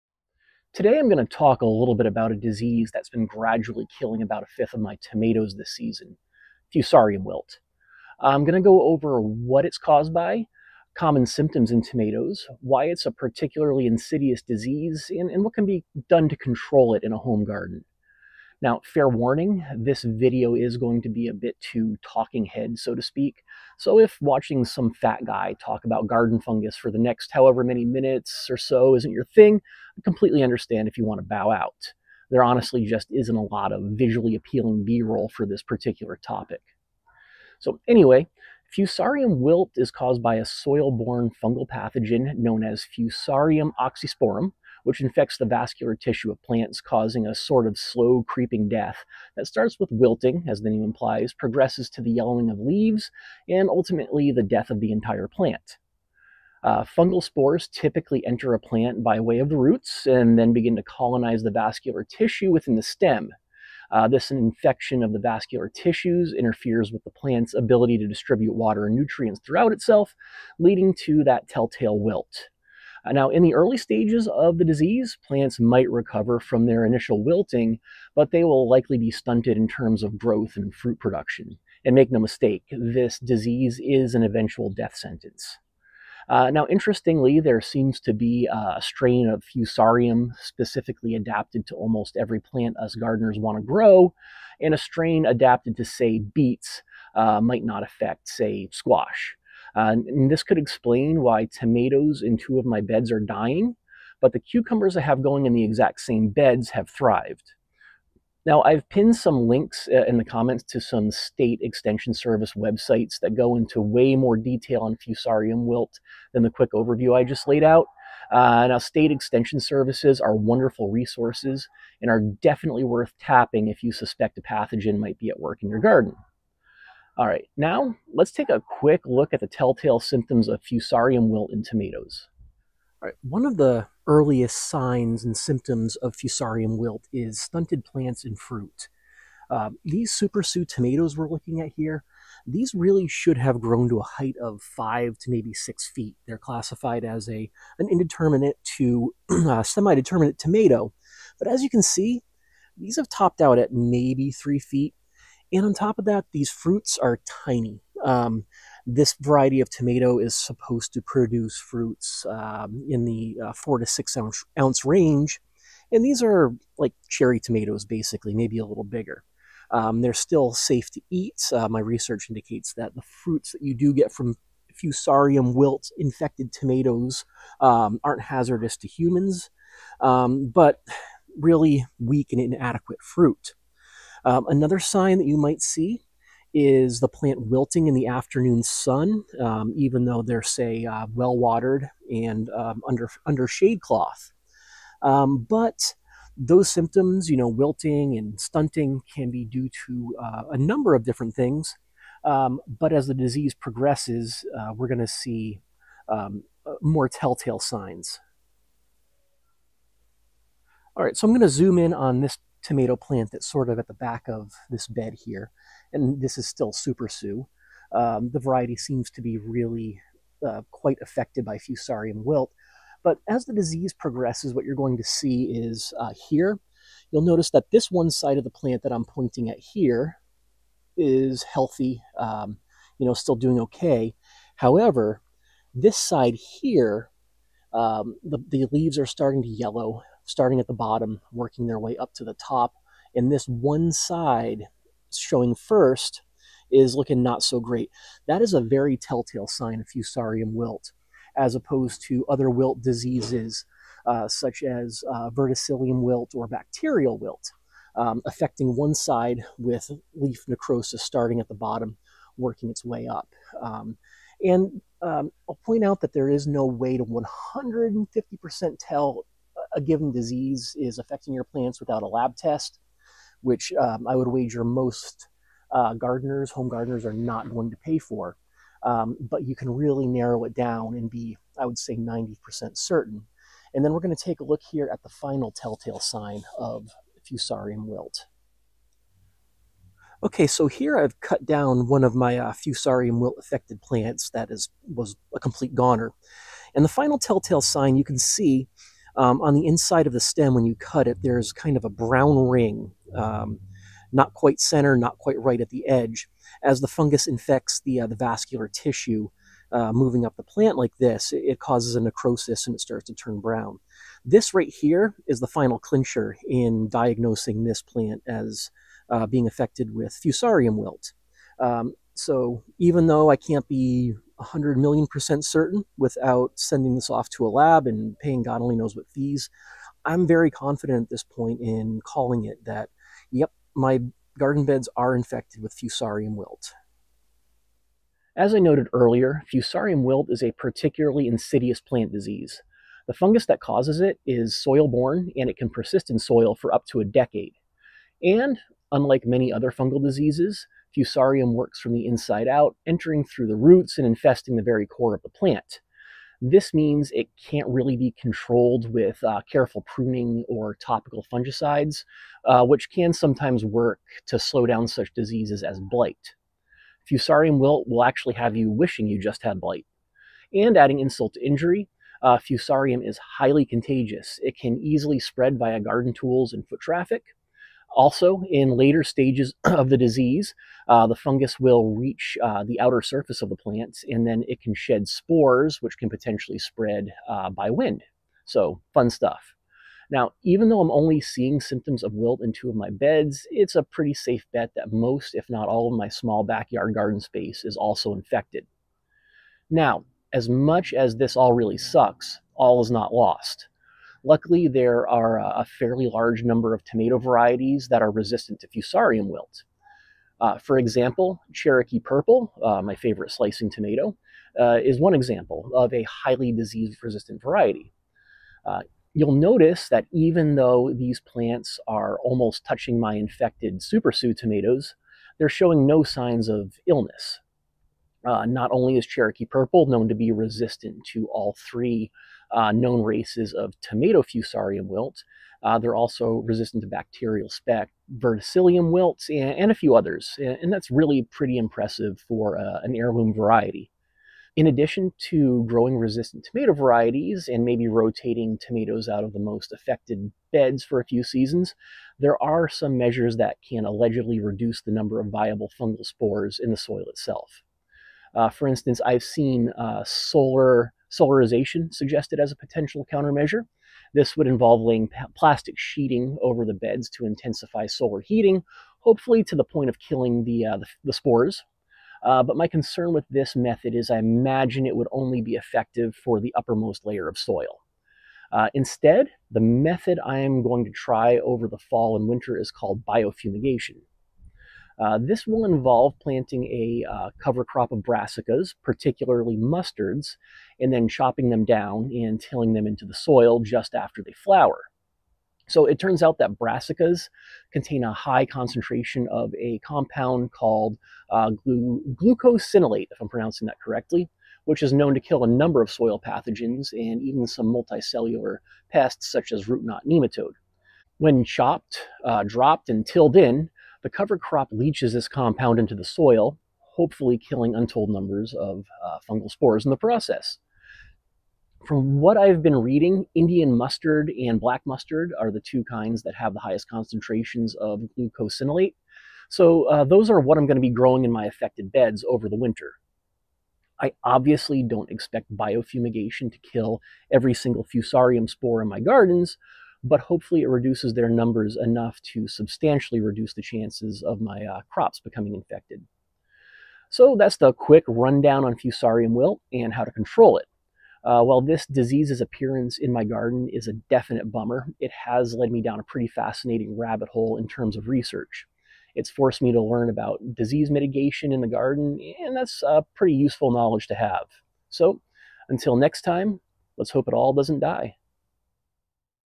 Fusarium Wilt in Tomatoes: Symptoms and Solutions
I have Fusarium Wilt gradually killing the tomatoes in two of my raised beds. This video goes over what fusarium wilt is, how to recognize it in your garden, and what you can do to mitigate it.